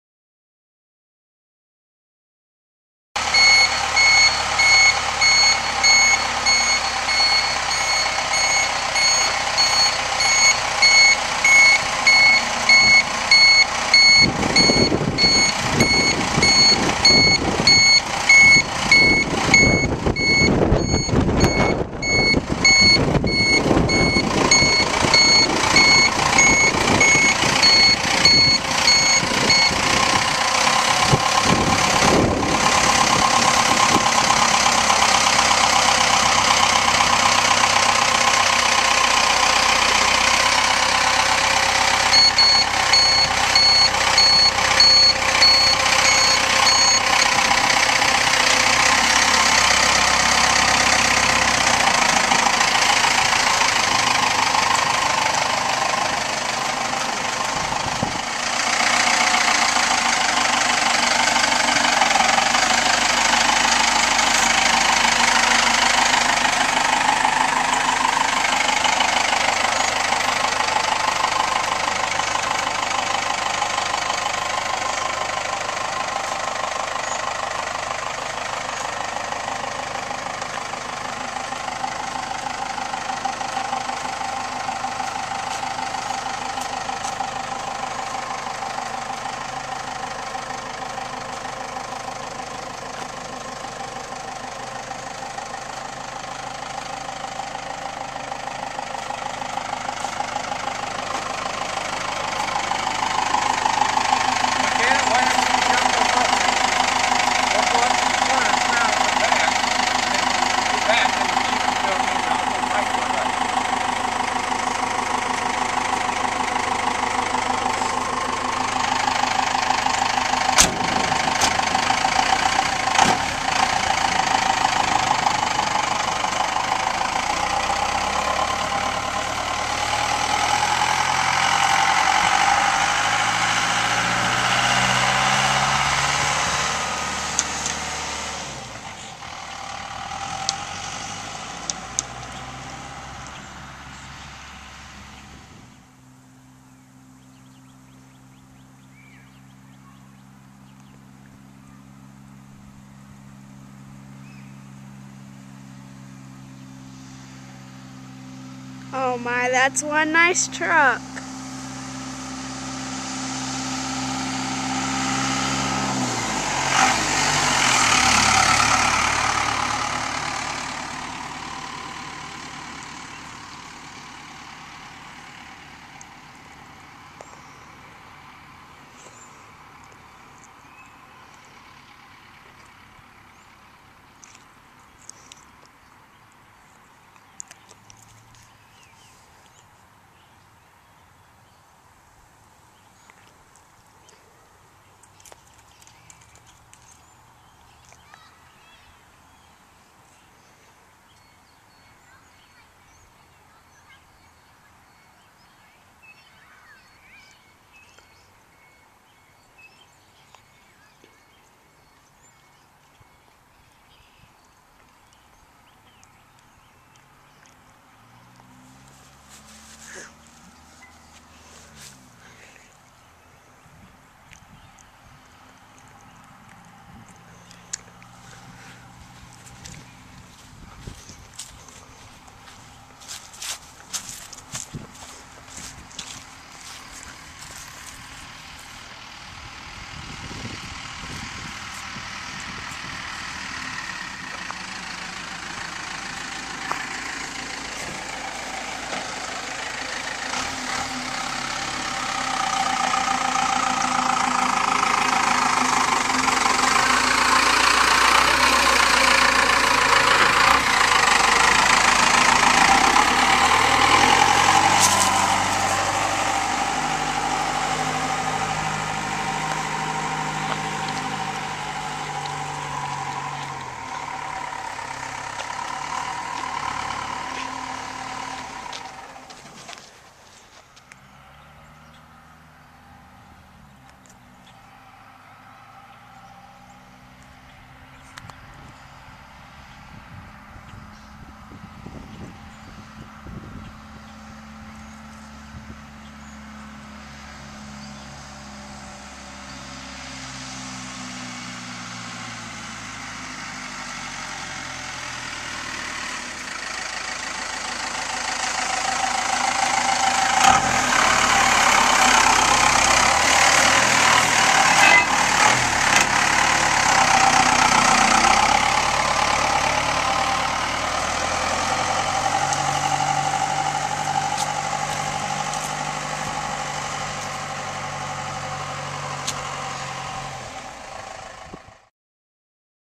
48 chevy engine swap -4bt cummins-twin stacks-3.07 -
48 chevy 4bt cummins-87 dodge rear end 3.07 disc brakes-4 speed-7 foot 10 inch twin stacks-10 foot box-78 chevy chassis,lime green,spoiler-4-53 two stroke swapped fro -4bt Cummins